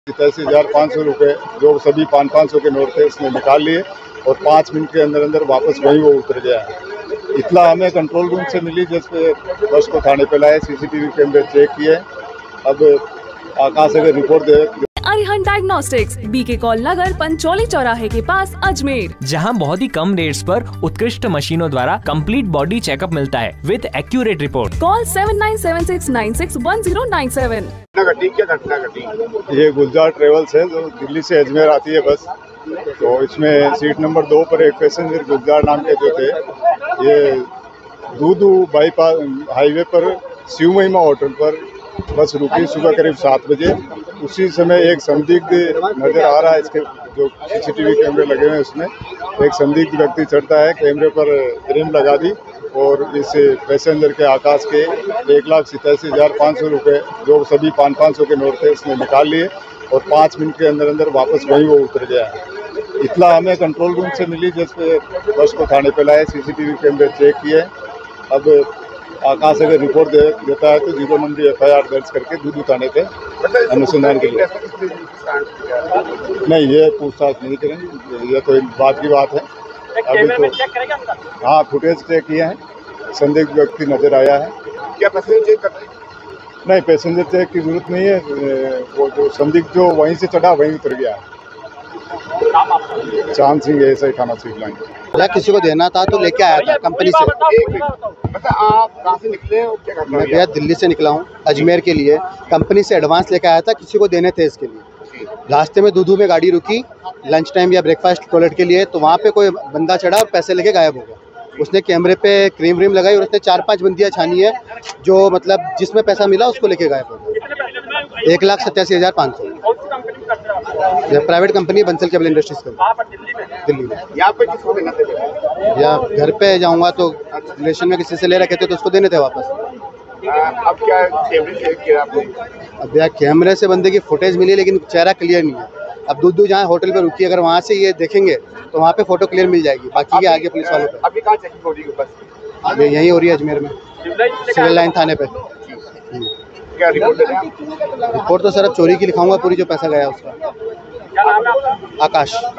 Ajmer | दिल्ली से अजमेर आ रहे युवक के पैसे हुए चोरी, युवक ने कहा दूदू में आधा घंटा रु | AYN NEWS
दिल्ली से अजमेर आ रहे युवक के पैसे हुए चोरी, युवक ने कहा दूदू में आधा घंटा रुकी थी बस वहां हुए चोरी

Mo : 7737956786